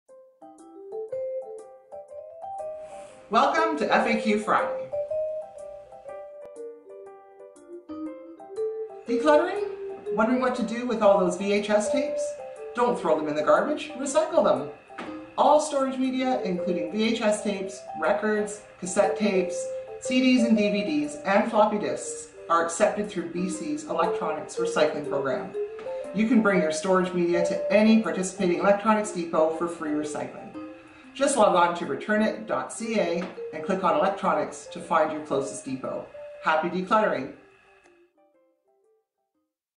FAQ Friday Ep 9: How to recycle VHS tapes
Do you have a VHS collection gathering dust at home?  We've got a solution for you on this week's FAQ Friday.